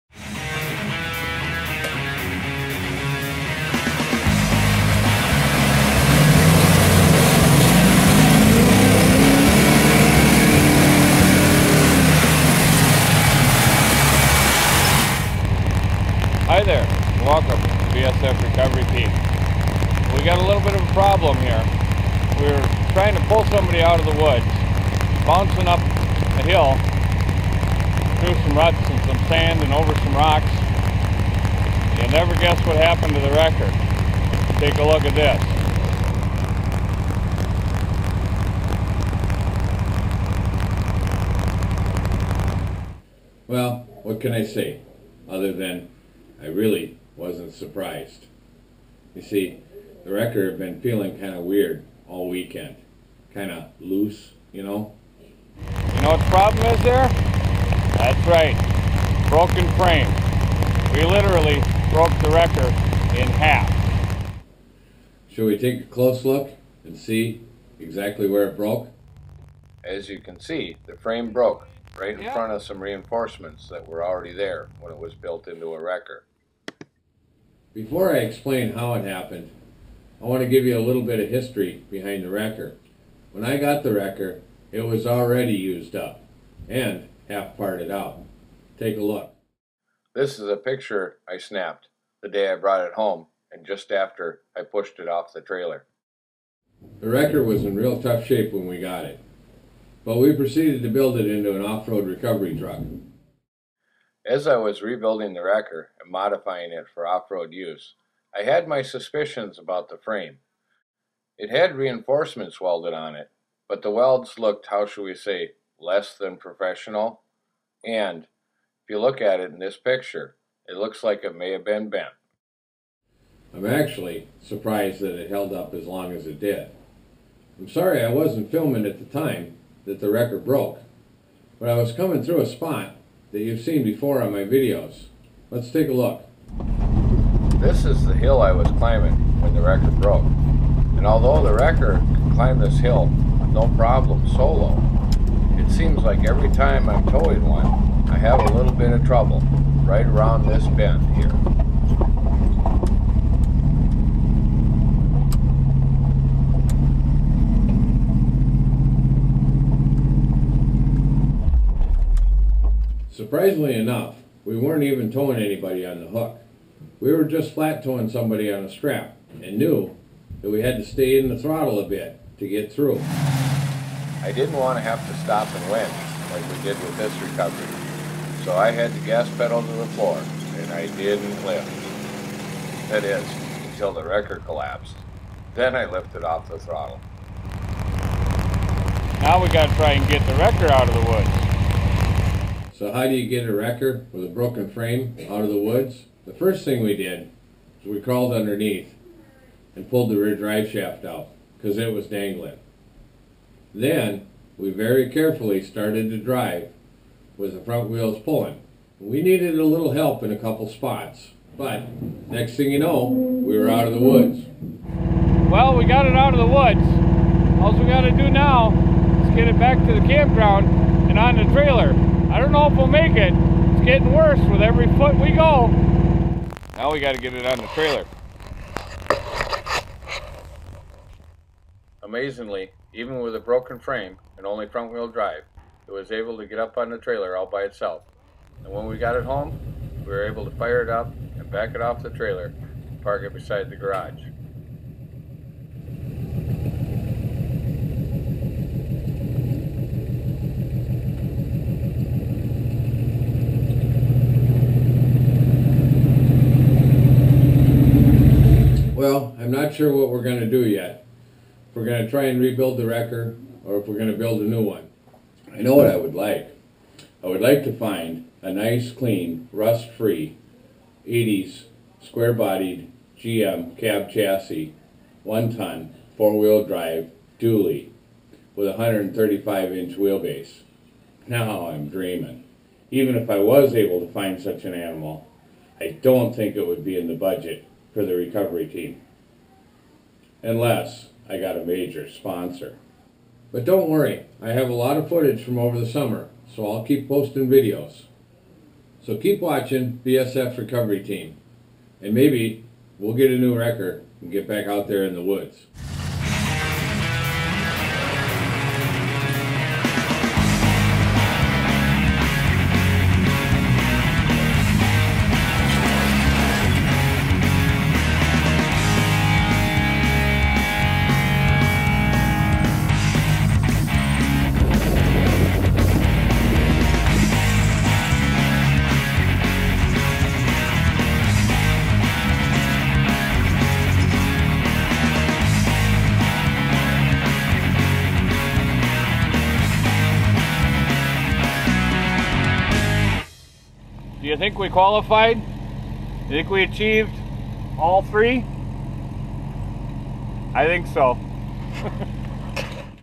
BAD DAY FOR THE WRECKER by BSF Recovery Team
The wrecker fails big time!